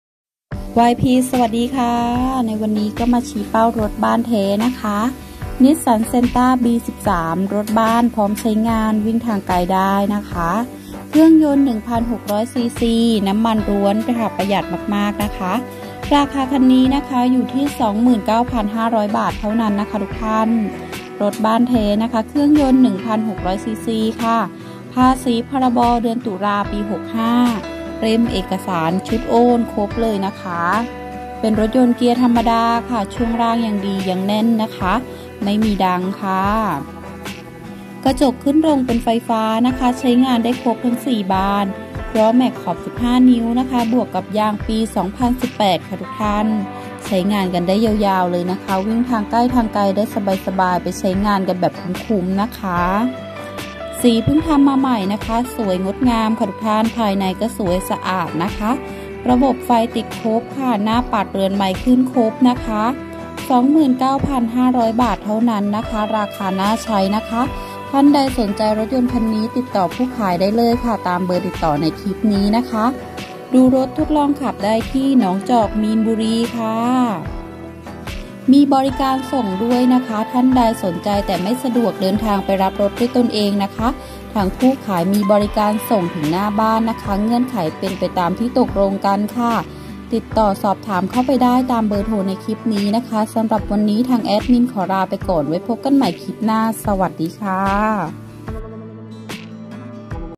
👉❌️คันนี้ขายแล้ว ❌️👈รถบ้านพร้อมใช้💥ขับทางไกลได้สบาย👍ราคาถูกกว่ามอเตอร์ไซค์
(นิสสัน เซนทร้าB13)
ขายรถบ้านใช้งานทางไกลได้100%
เครื่อง1,600ccน้ำมันล้วนโครตประหยัด
ภาษีตุลาคม65+เล่มเอกสารชุดโอนครบ
เกียธรรมดา+ช่วงล่างยังแน่นไม่มีดัง
กระจกไฟฟ้าขึ้น-ลงครบ4บาน
แมคขอบ15นิ้ว+ยางปี18ทั้งชุดใช้อีกนาน
สีพึ่งทำมาใหม่สวย+ภายในสวยมาก
ระบบไฟติดครบ+หน้าปัดเรือนไมล์ขึ้นครบ
มีบรีการส่งจริง08399xxxxดูรถได้หนองจอกมีนบุรี
ราคา29,500บาท.